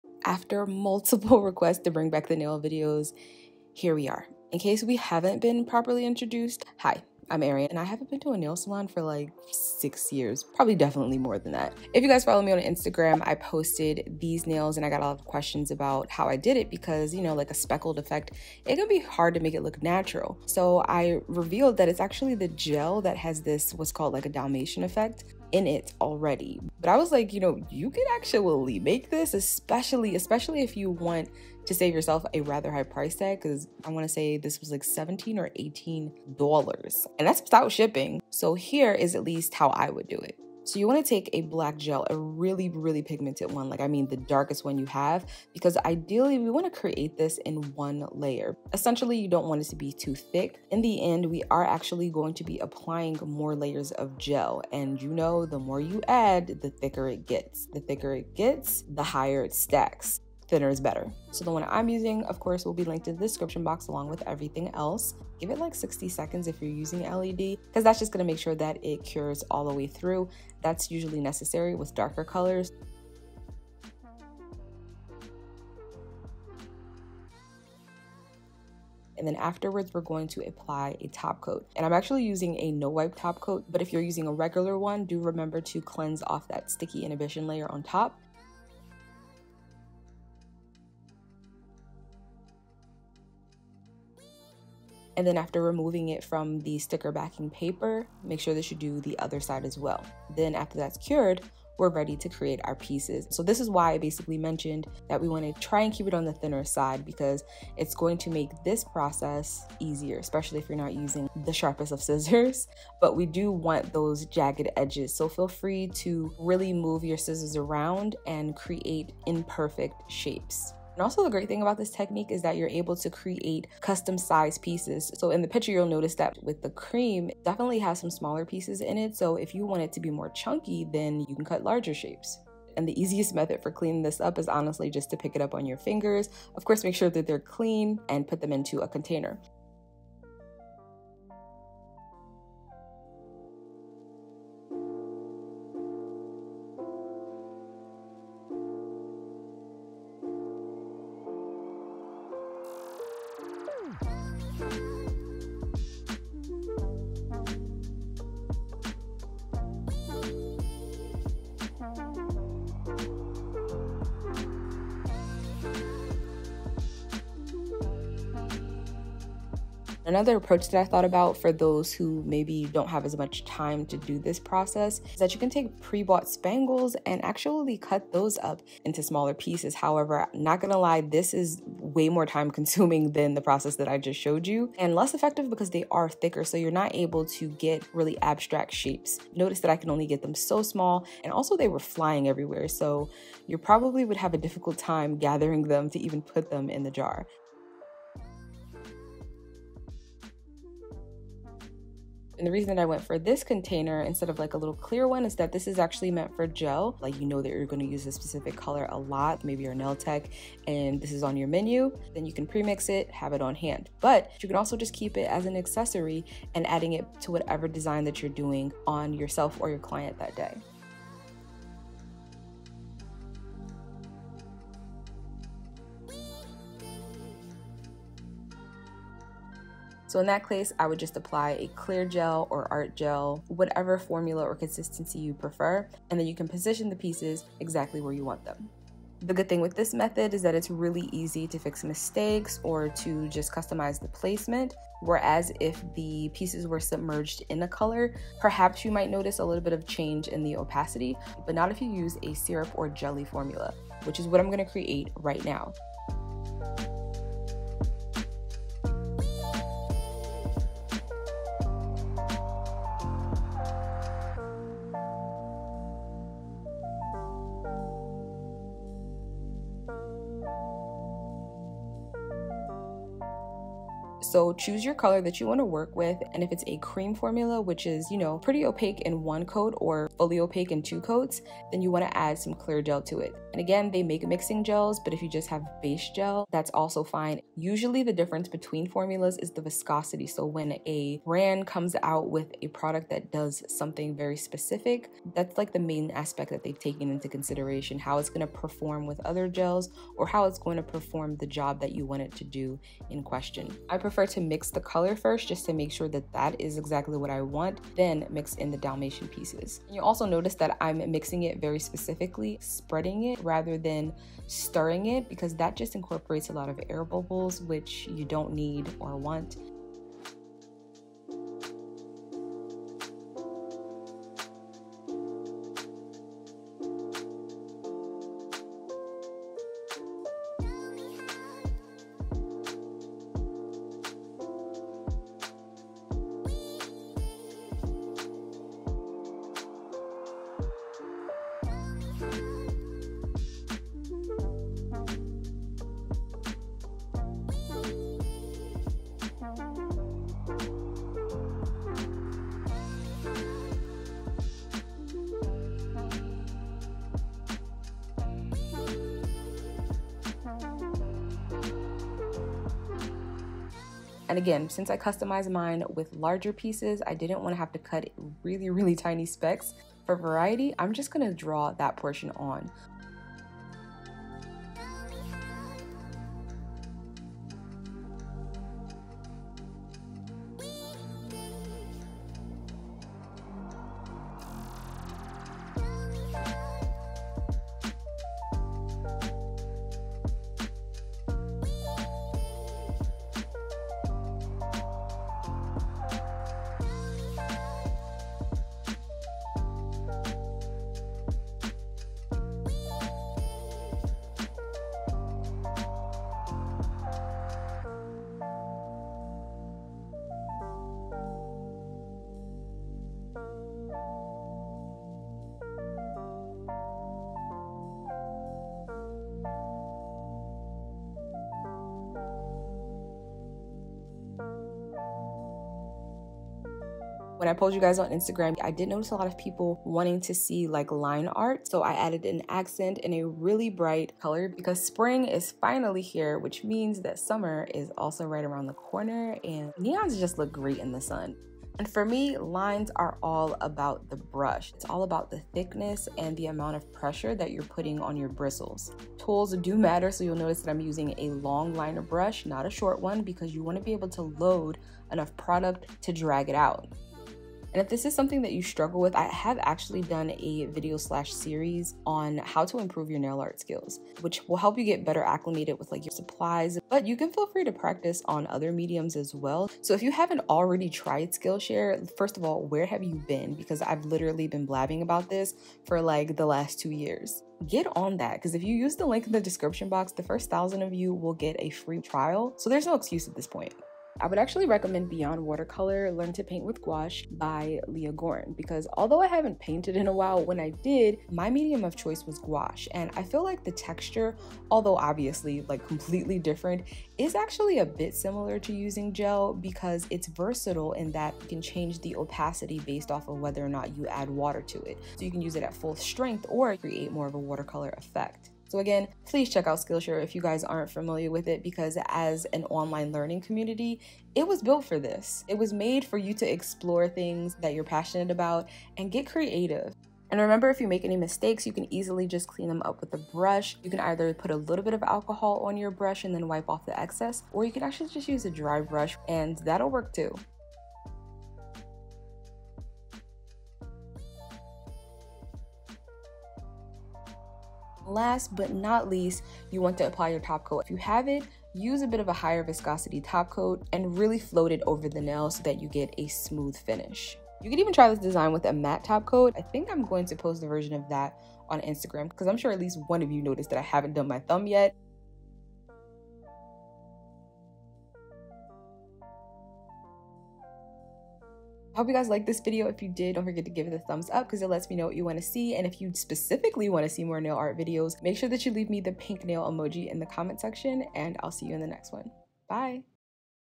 QUARANTINE SELF CARE | DIY Dalmatian / Terrazzo Gel Nail Art Tutorial 달마시안 젤
SUPPLIES
estemio gentle base gel
nail partner long oval brush
estemio better black
estemio no wipe top gel
vetro color gel #02
vetro clear fuji base gel (mixing)
lucu oval gel brush 7.5
vetro color gel #305
leaf gel color #202
ace gel hot pink
hana4 long liner brush
vetro top gel
lucu flat gel brush 10.5
led nail lamps ( get $10 0ff)

RECAP
0:00 Intro
0:03 Base
0:41 DIY Dalmatian
2:56 Alternate DIY
3:33 Demo
4:35 Custom Dalmatian Gel
5:21 Application
6:11 Details
7:15 Line Art
8:56 Top Coat